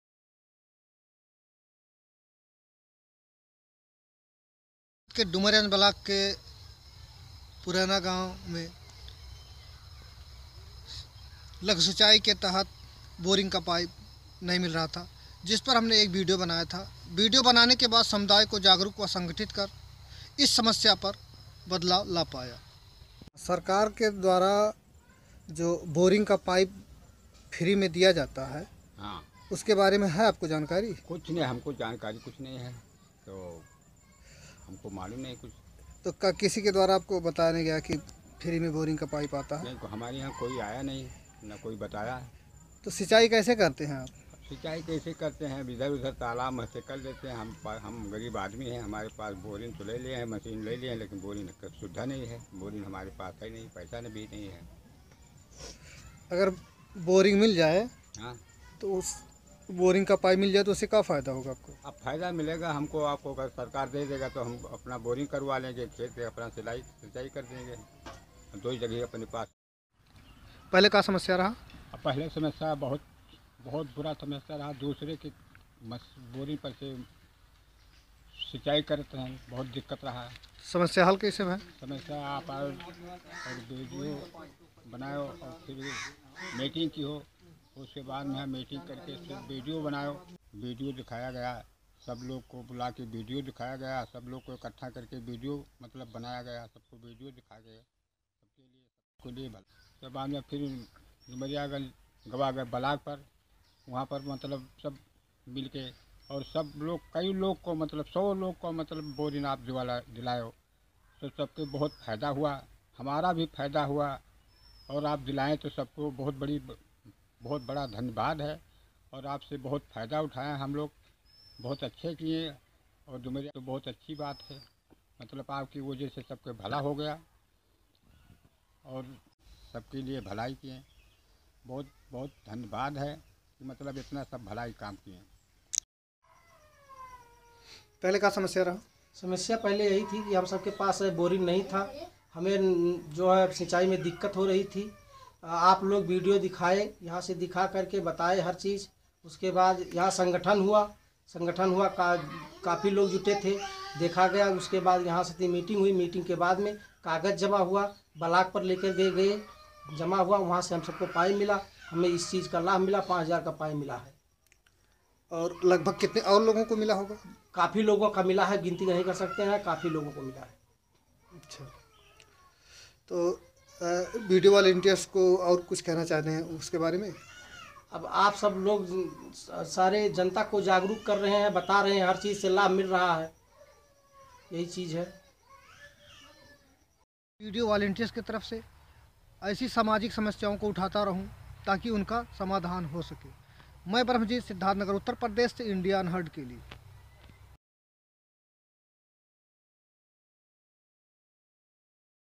IMPACT | 100 किसानो को मिला बोरिंग पाईप
उत्तर प्रदेश के सिद्धार्थ नगर जनपद के डुमरियागंज ब्लाक के पुरैना गांव मे सिंचाई करने मे बहुत परेशानियो का सामना करना पड़ता था जिस समस्या पर सामुदायिक संवाददाता ब्रह्मजीत ने एक वीडियो बनाया था| वीडियो बनाने के बाद समुदाय को जागरूक व संगठित कर इस समस्या पर बदलाव लाया| इस विडियो के कारण समुदाय के लगभग  100 किसानो को बोरिंग पाईप  मिला|

Community Correspondent Brahmajeet reports from Siddhartnagar, Uttar Pradesh for Video Volunteers.  Learn more about Brahmajeet and see other videos of this Community Correspondent